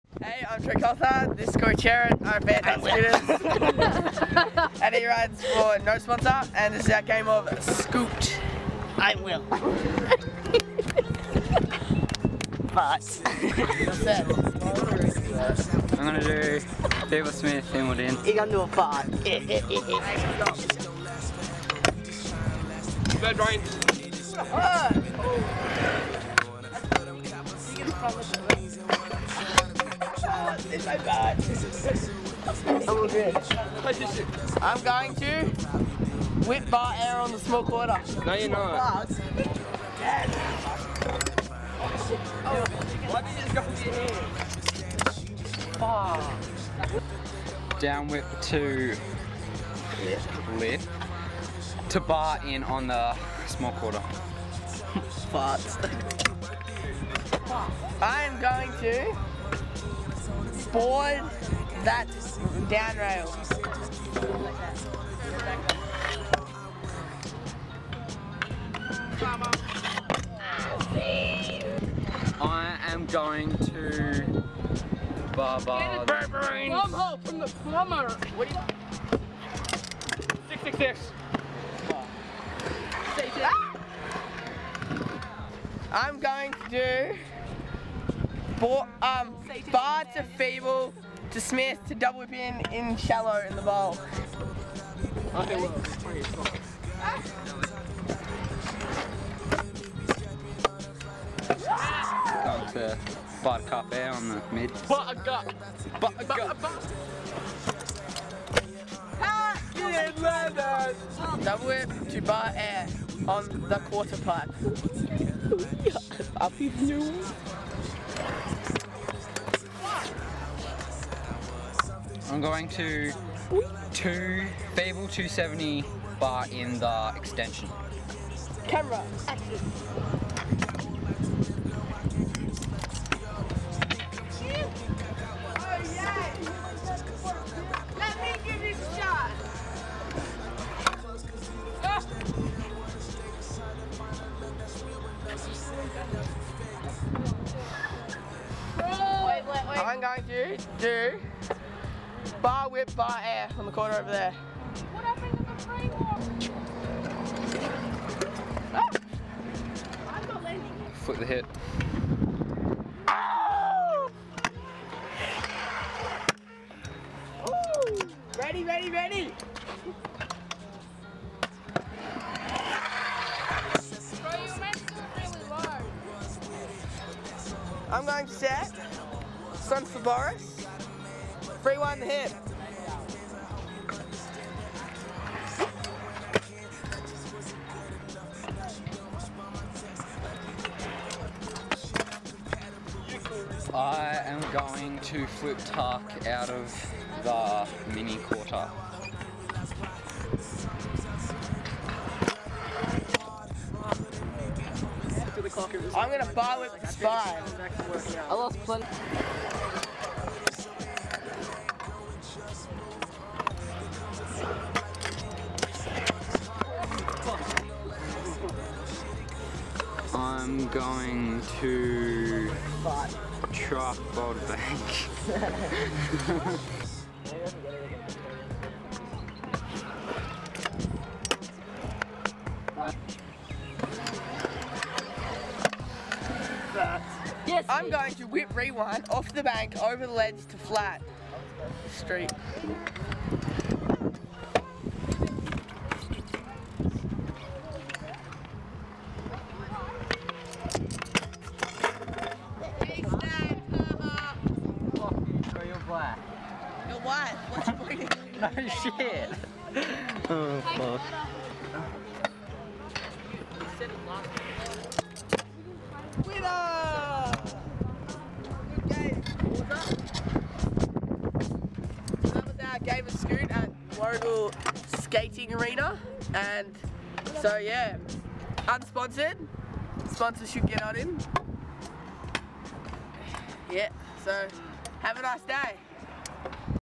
Troy Coulthard VS Corey Cherrett | S.C.O.O.T!
Troy came to warragul a couple of weeks ago and him and corey wanted to play a game of scoot which they wanted me to film and this is how it turned so i hope you's all like it, the filming and editing i pretty shit so please dont hate and im sorry about the distraction in the background.

Filmed & Edited By: Tyler Cherrett

P.S. this is not my fisheye Dylan Arldt let me borrow it :)